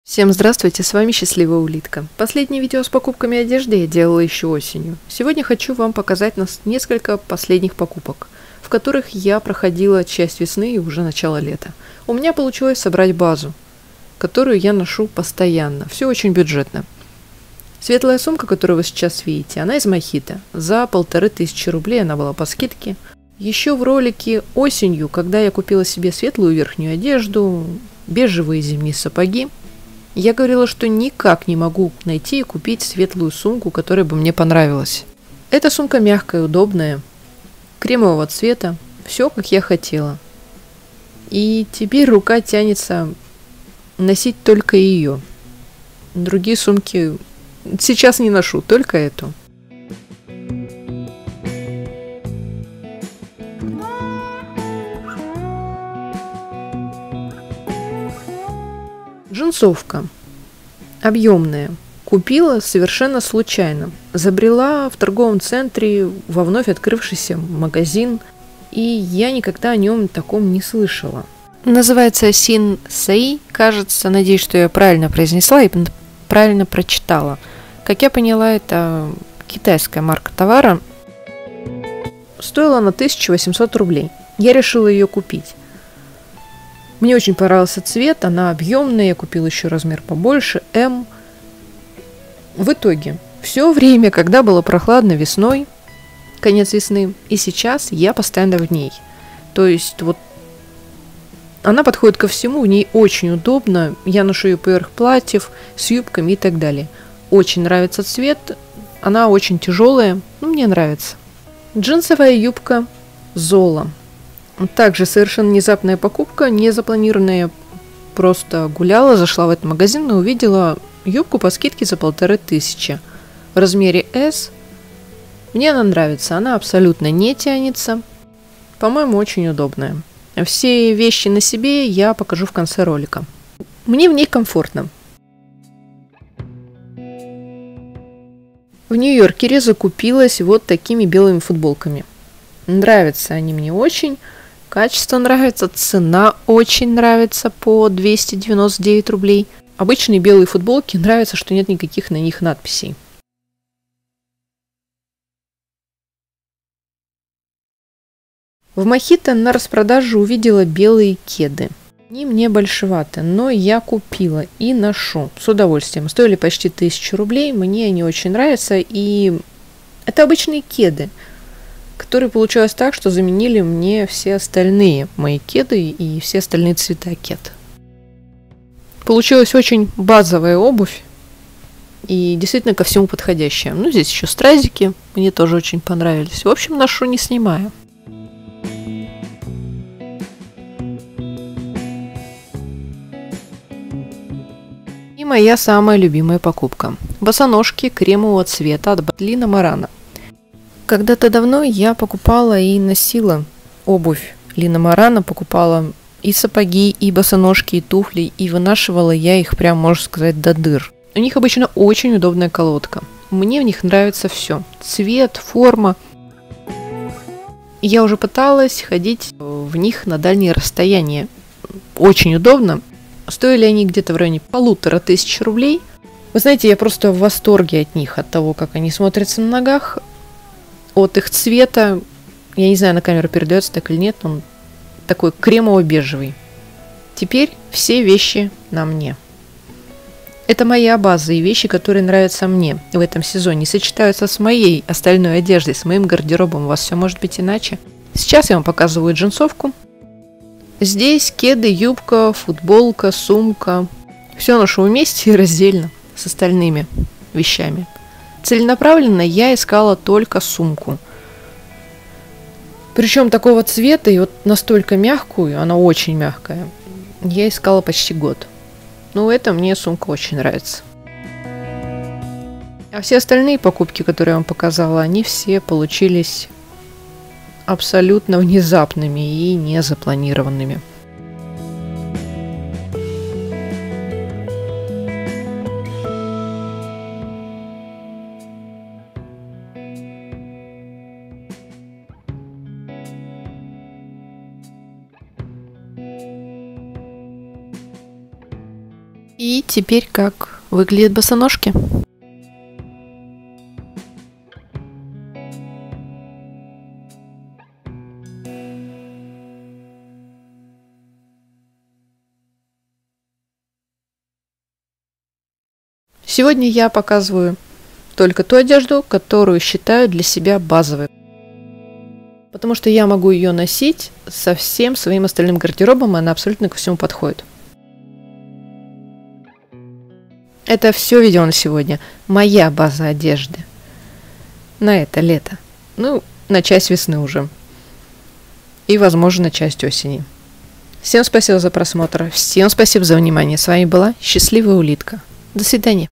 КУПИЛА БАЗОВУЮ БЮДЖЕТНУЮ ОДЕЖДУ НА ЛЕТО. ЦЕНЫ И ПРИМЕРКА.
Купила базовый гардероб. Удобная повседневная одежда.
Цены и примерка.
Все бюджетно.